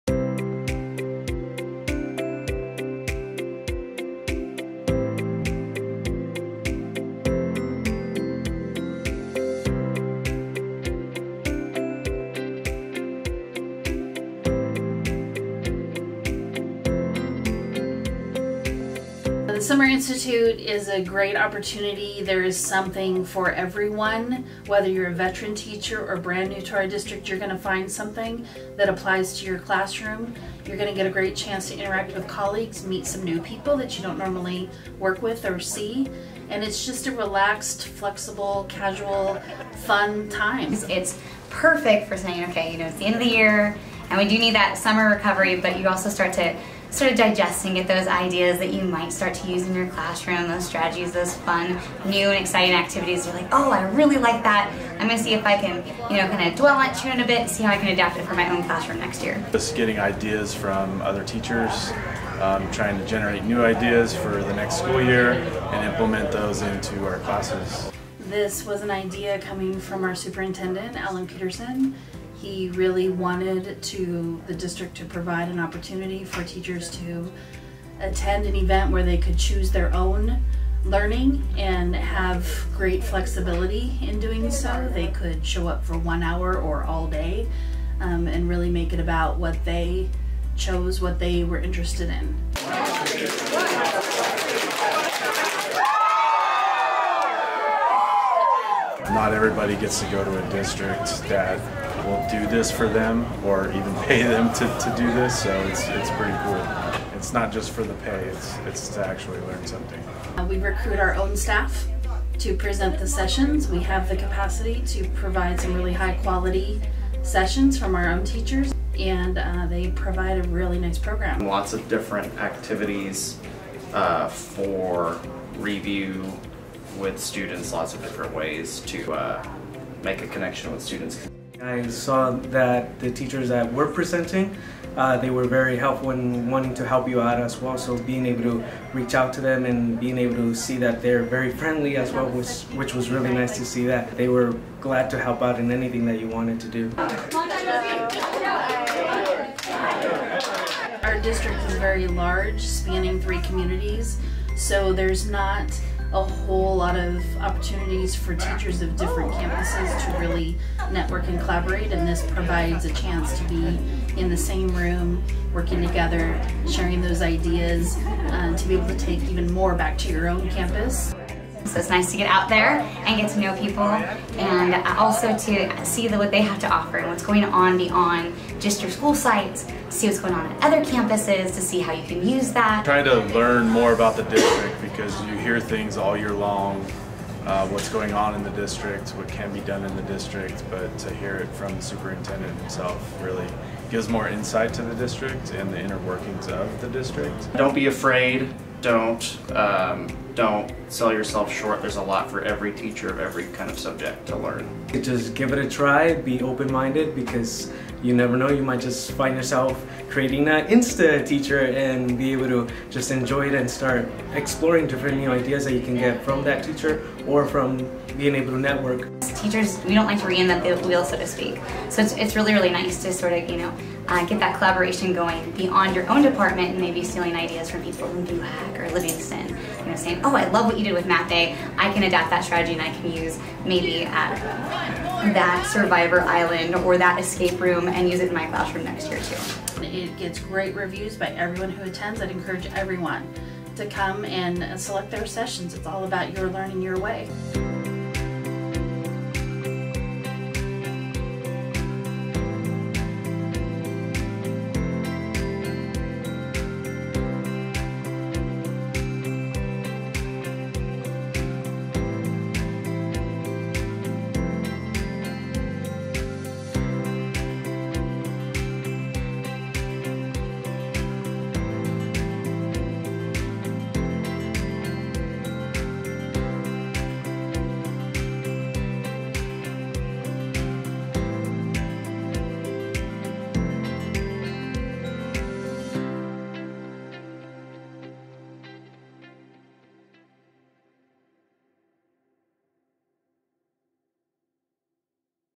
MUHSD Summer Institute
MUHSD Summer Institute is a series of workshops put on by the district that provides an opportunity for teachers to sharpen their classroom skills, network with other teachers and explore new ways to keep students excited about learning.

The first session was held the week after school let out. A second session is scheduled for the last week of July.